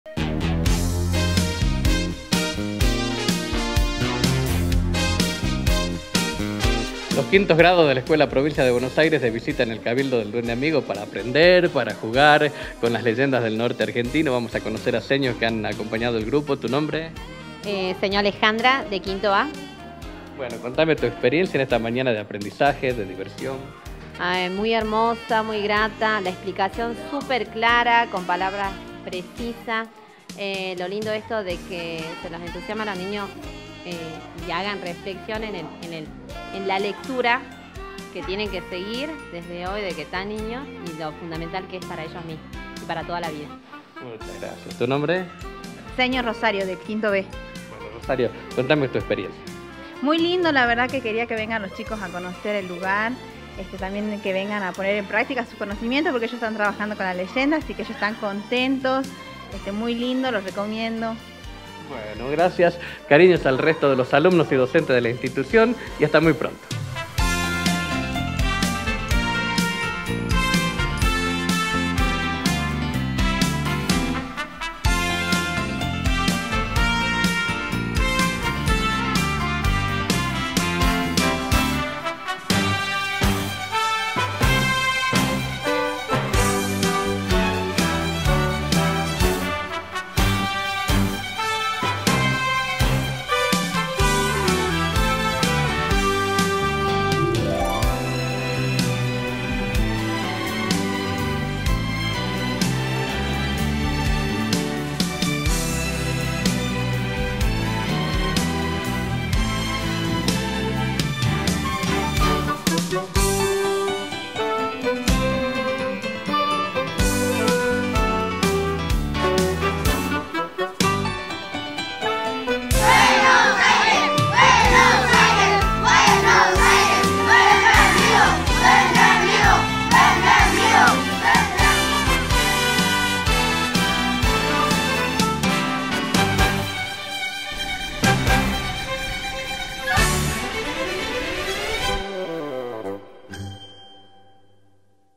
Visita de 5º de la Escuela Provincia de Buenos Aires al Duende amigo - 24 de junio de 2024
Los integrantes de 5º de la Escuela Provincia de Buenos Aires (Salta) visitaron el Cabildo del Duende amigo de La Silleta el 24 de junio de 2024 ¡¡Gracias por hacernos pasar un momento tan especial!! Si querés venir con tus alumnos comunicate al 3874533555.